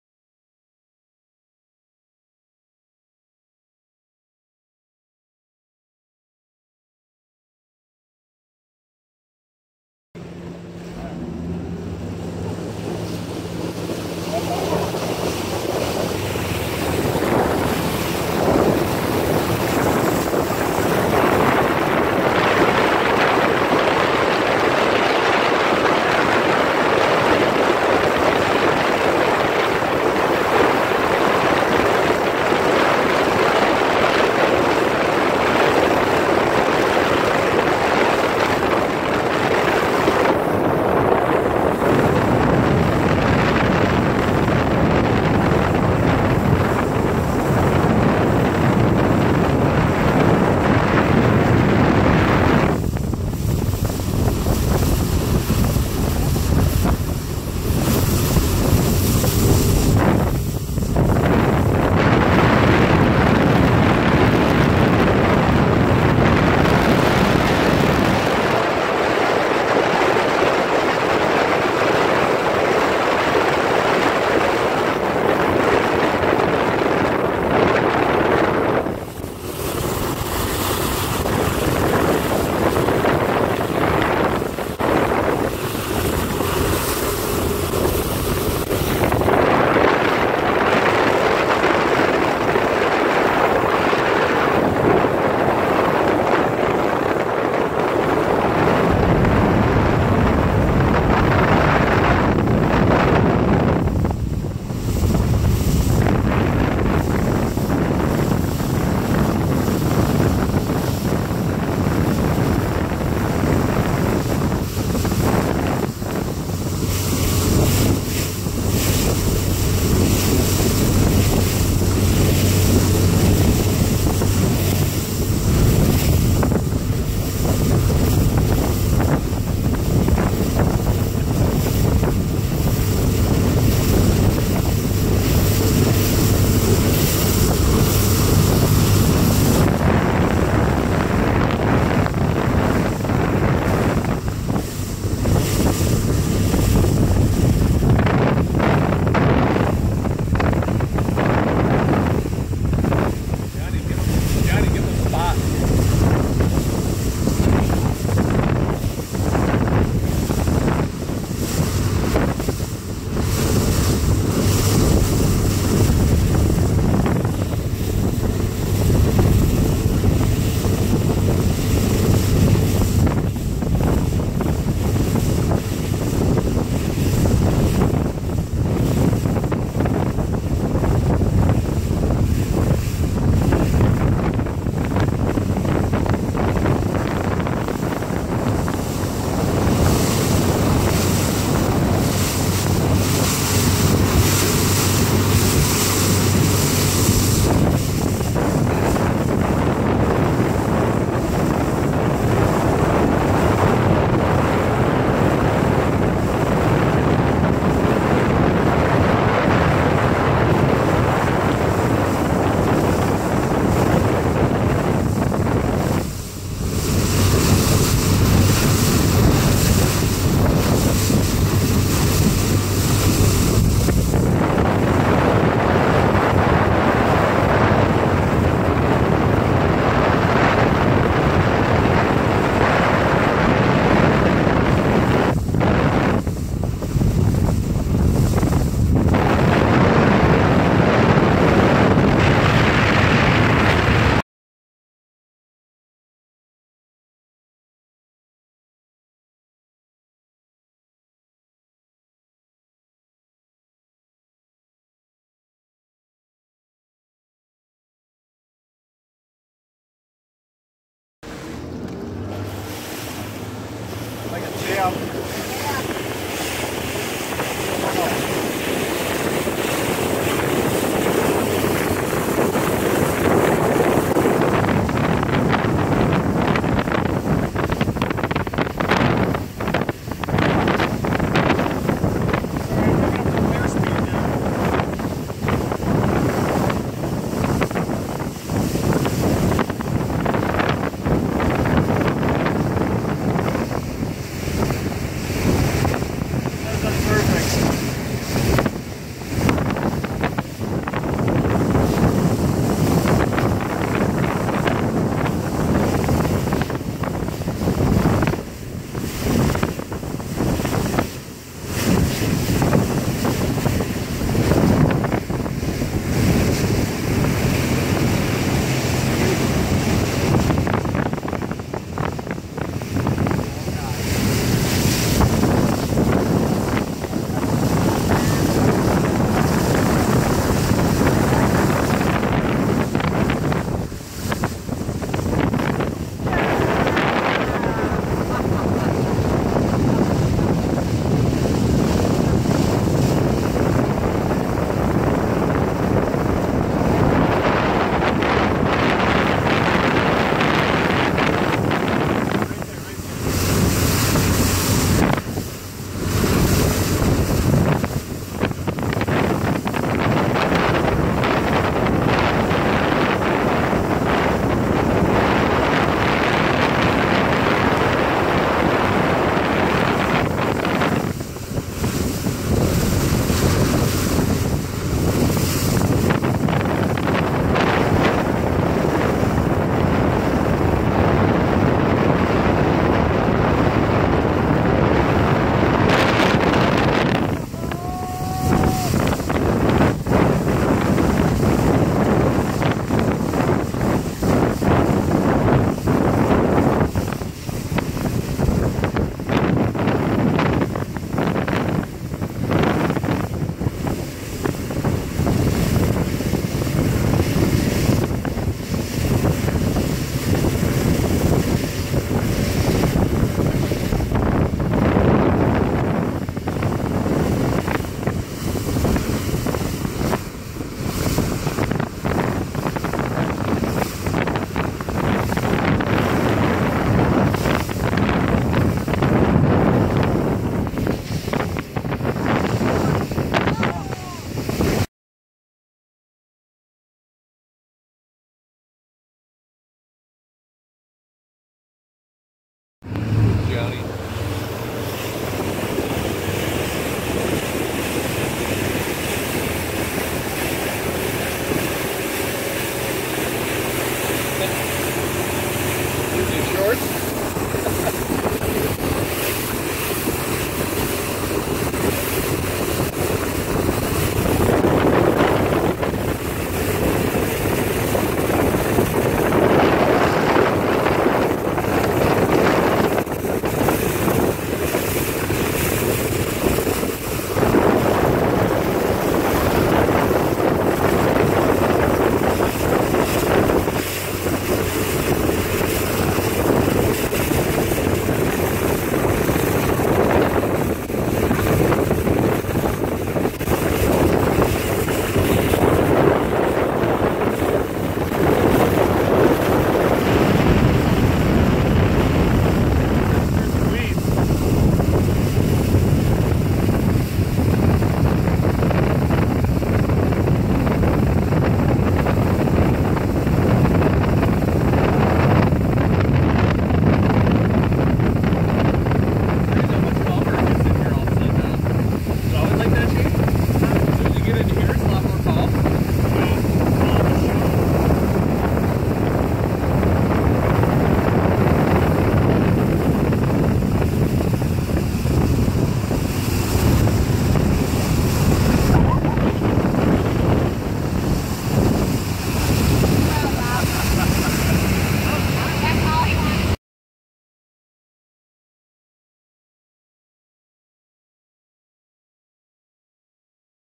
Fourth of July 2017. Skiing on Lake Latoka
My cousin Shane and his wife Lori and their awesome kids, Bailey and Bella had friends and family over to their beautiful place for a Fourth of July party.  The day began with us watching a cool boat parade.  After the parade a bunch of us adults had a water fight with the kids.  SO MUCH FUN!!!  After that was fun for all, including kayaks,  paddle boats, water trampoline, and other outdoor fun.  The two daredevils on the skis were Blair George who was on the water first and next was Jon "Sizzle" Crouse.  Very impressive skiing skills!!